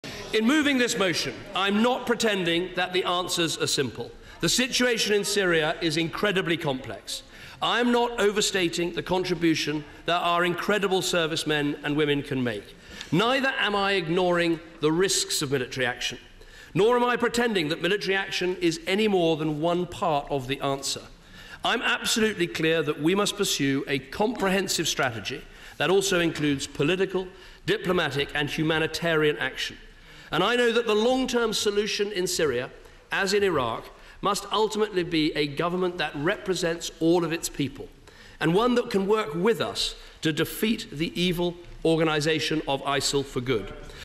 Cameron: Military action is only 'one part of the answer' - BBC News
David Cameron said in the opening of the debate on airstrikes in Syria that they would only be "one part of the answer". He acknowledged that the situation is "incredibly complex".
However he insisted that he was "not ignoring the risks" of undertaking military action in the country.